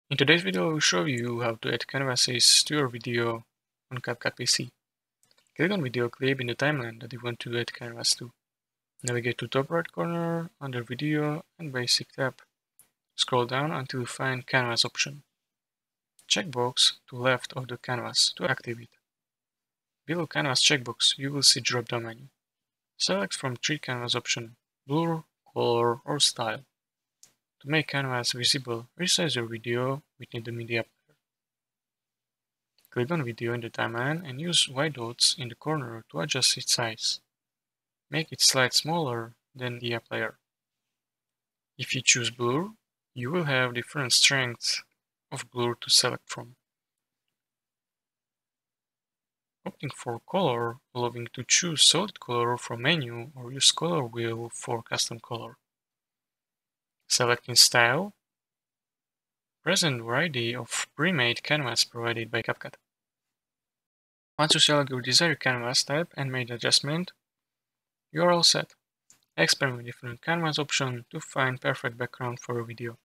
How to Add Canvas In Video On CapCut PC (2025)
Today, I'll guide you through the process of adding canvases to your videos using CapCut on PC. Canvases serve as backgrounds for your videos, enhancing various editing styles. Here's how to do it:

1. **Accessing Canvas Options:**
   - Click on the video clip in the timeline that you want to add a canvas to.
   - Navigate to the top right corner, under the "Video" and "Basic" tabs.
   - Scroll down until you find the "Canvas" option.

2. **Enabling Canvas:**
   - Check the box to the left of "Canvas" to activate it.

3. **Choosing Canvas Type:**
   - Below the canvas checkbox, you'll see a dropdown menu.
   - Select from three canvas options: blur, color, or style.

4. **Resizing Video:**
   - To make the canvas visible, resize your video within the media player.
   - Click on the video in the timeline and use the white dots in the corner to adjust its size. Make it slightly smaller than the media player.

5. **Customizing Canvas:**
   - If you choose "Blur," you'll have four different strengths of blur to select from.
   - Opting for "Color" allows you to choose a solid color from the menu or use the color wheel for a custom color.
   - Selecting "Style" presents a variety of pre-made canvases provided by CapCut.

6. **Final Steps:**
   - Once you've selected your desired canvas type and made adjustments, you're all set.
   - Experiment with different canvas options to find the perfect background for your video.

7. **Conclusion:**
   - Adding canvases to your videos can significantly enhance their visual appeal and style.